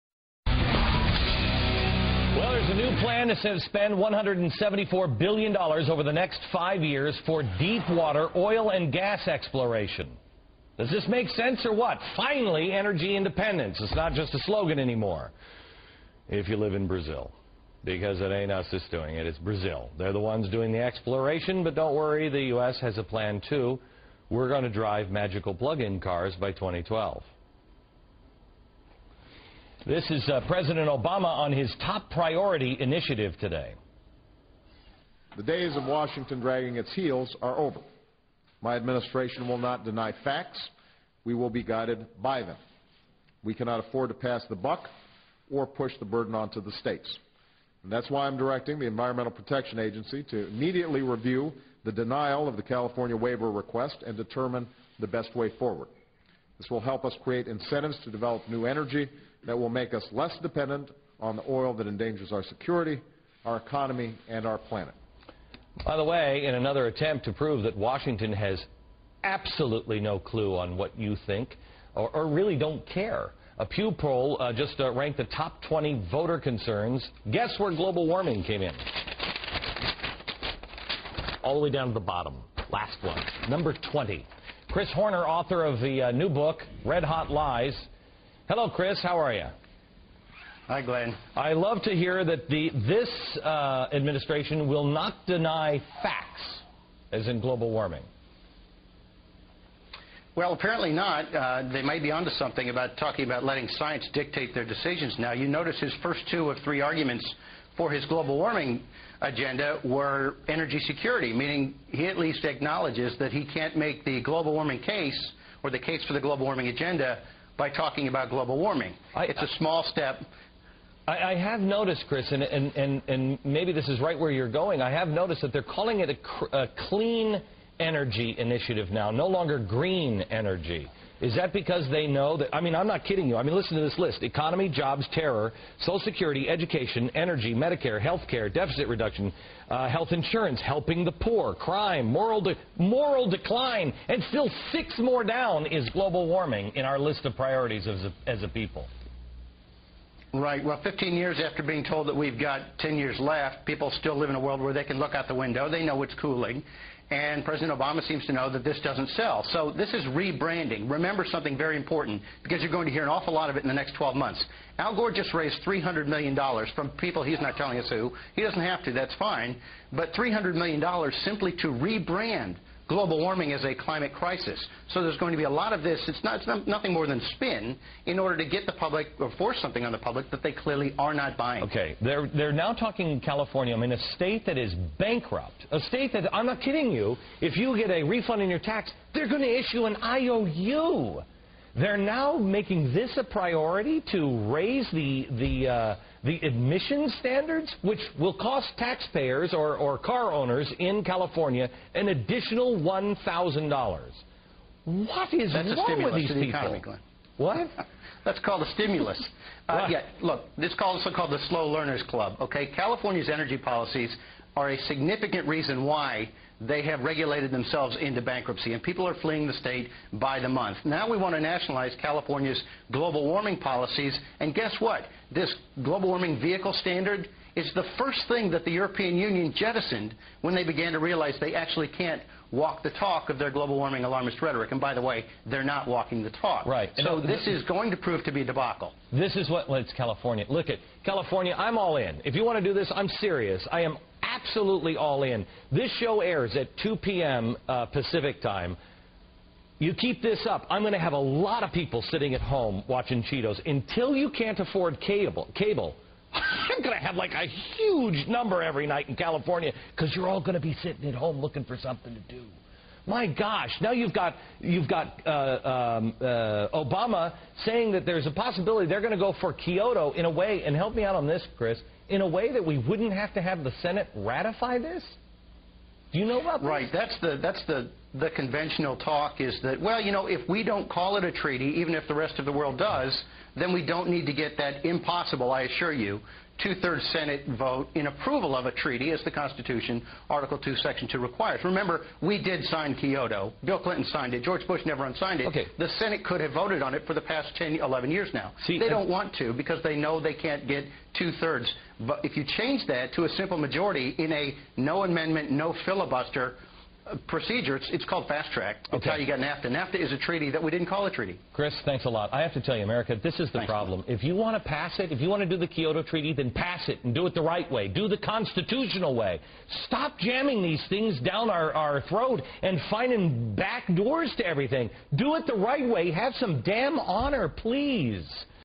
Chris Horner on President Obama's Global Warming Policy
Competitive Enterprise Institute Senior Fellow Christopher C. Horner critiques President Obama's global warming policy.